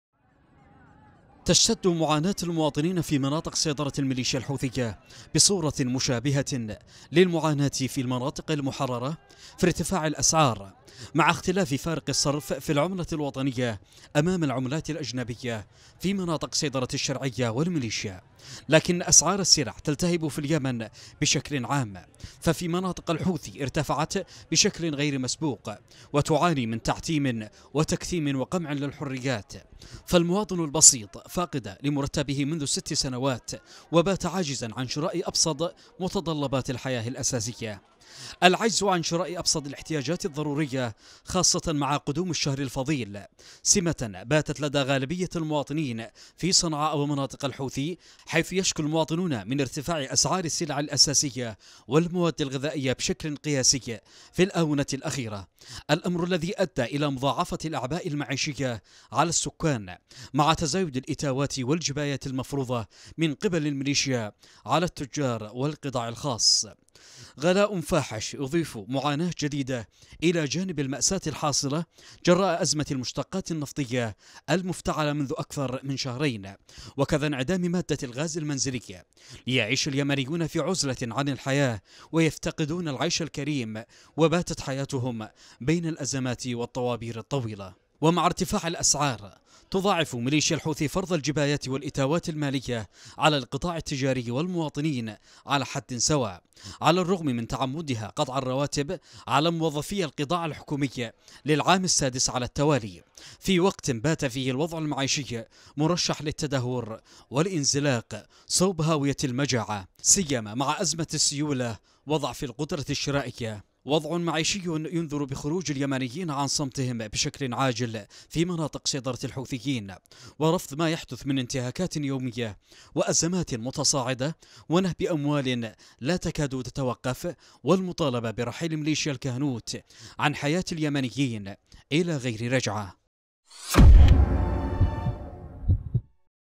أسواق صنعاء تشتعل بلهيب الأسعار
تلتهب أسواق صنعاء بشكل غير مسبوق في مناطق سيطرة ميليشيات الحوثي في ظل عجز المواطن عن شراء أبسط الاحتياجات الاساسية مع قدوم شهر رمضان المبارك
ـــــــــــــــــــــــــــــــــ
 تقرير أكرم  سران

تليجرام